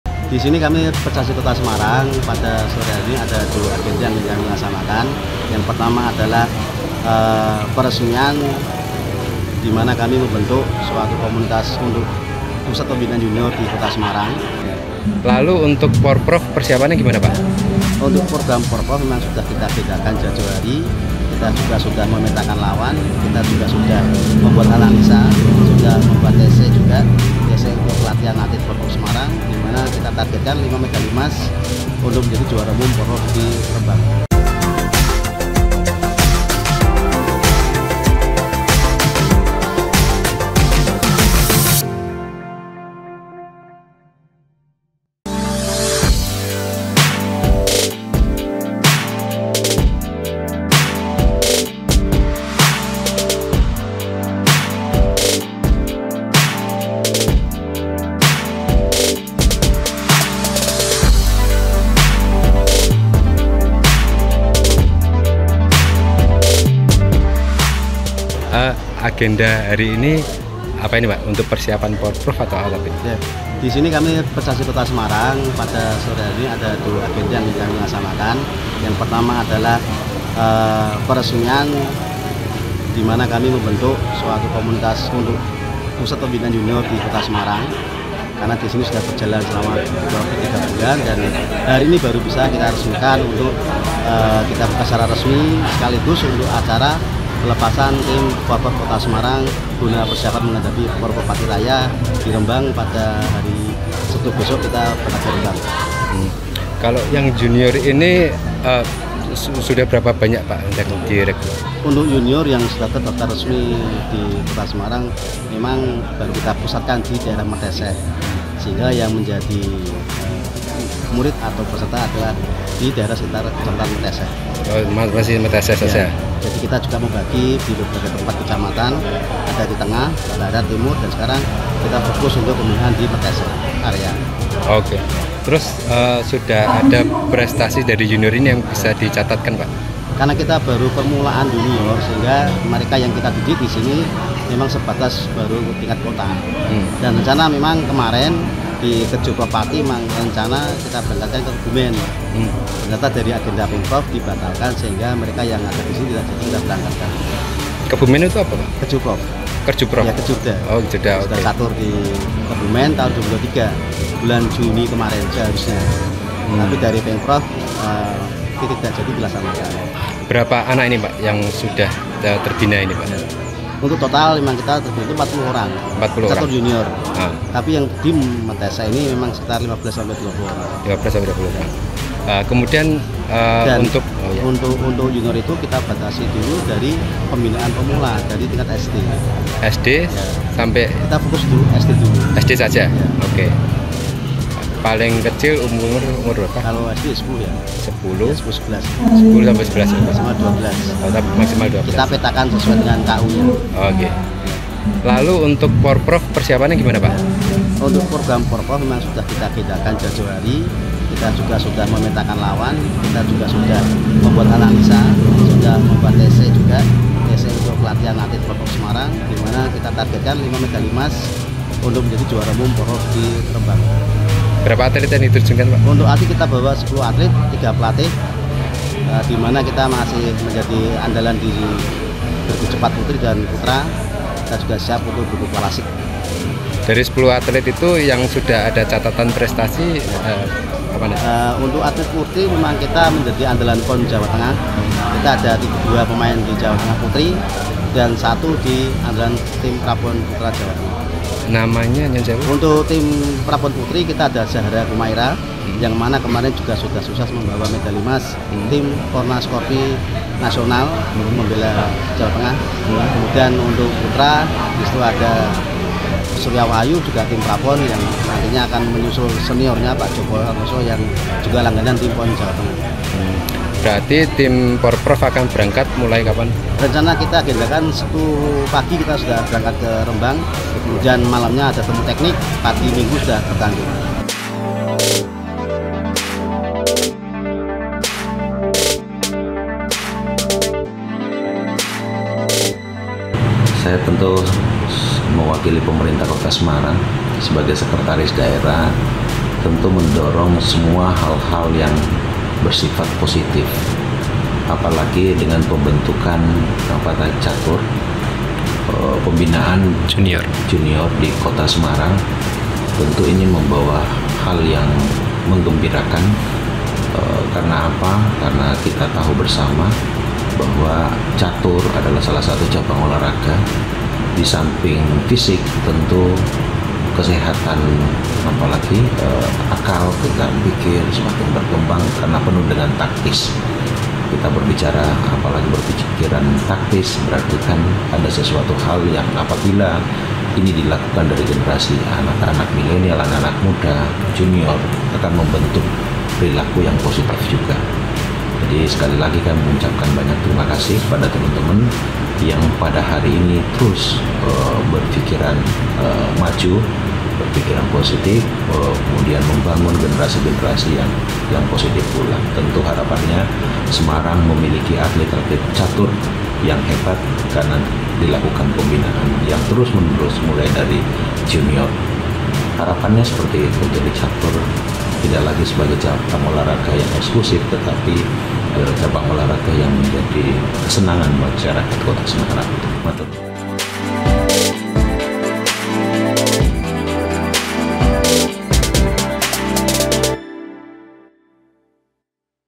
Percasi Kota Semarang Targetkan 5 Emas di Porprov Jateng 2023
BETANEWS.ID, SEMARANG - Persatuan Catur Seluruh Indonesia (Percasi) Kota Semarang menargetkan lima medali emas untuk jadi juara umum catur dalam Pekan Olahraga Provinsi (Porprov) Jateng ke-XVI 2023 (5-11 Agustus) mendatang.

Ketua Percasi Kota Semarang, Didik Sakti Aji, mengatakan, untuk meraih target itu, Percasi terjunkan 10 atlet yang 2 di antaranya sudah punya prestasi.